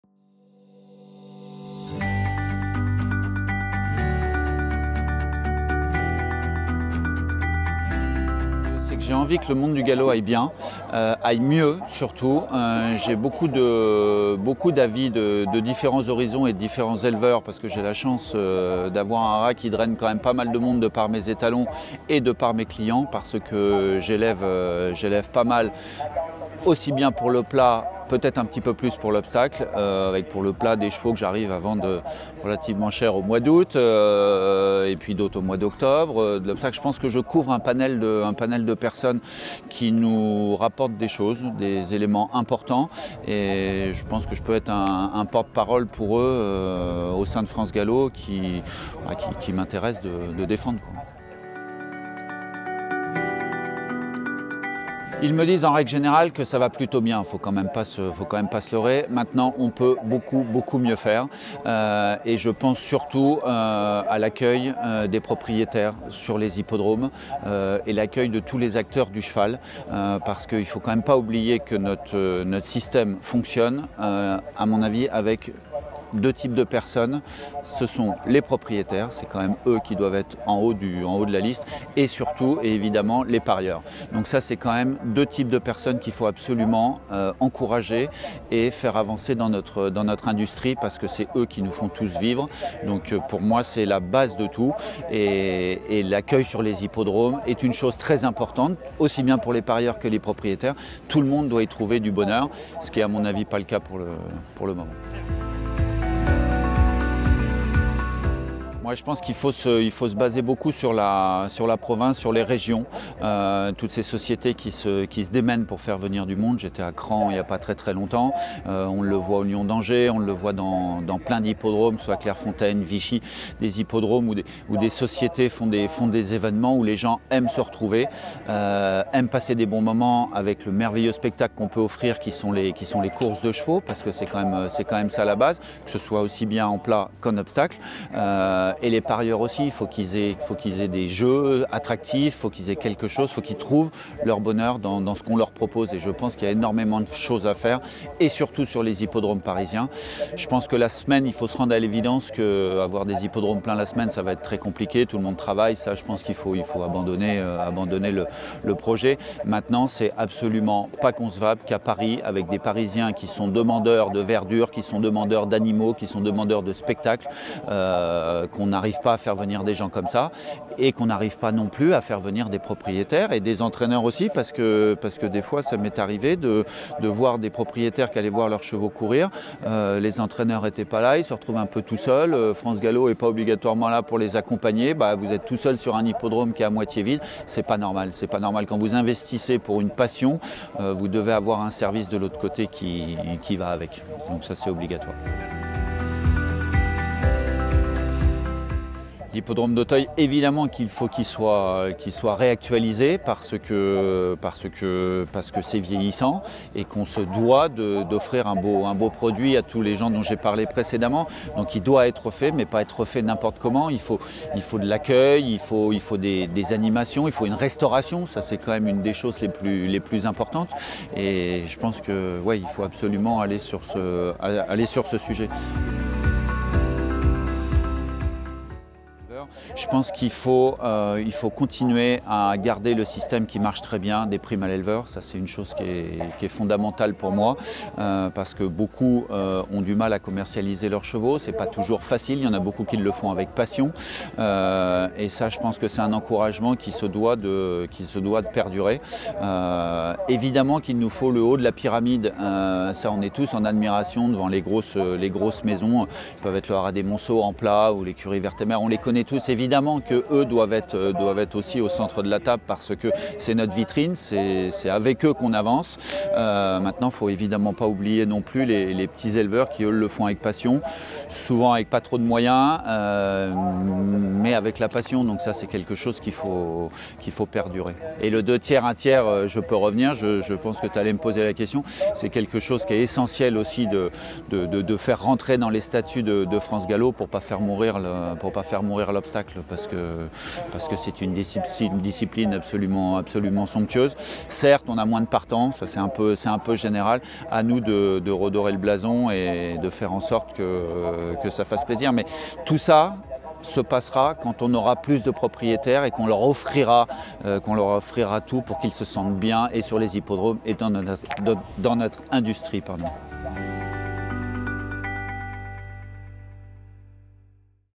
Elections 2023 - Anthony Baudouin, tête de liste des éleveurs d'Alliance Galop
Engagé pour la 1e fois à la tête de la liste des éleveurs présentée par le groupe Alliance Galop, Anthony Baudouin connait parfaitement l'élevage de plat et d'obstacle puisqu'il dirige le Haras du Hoguenet en Normandie, qui présente une activité mixte. Il souhaite focaliser les efforts de la prochaine mandature sur le cas des propriétaires, qui sont la pierre angulaire de la santé de toute l'industrie des courses.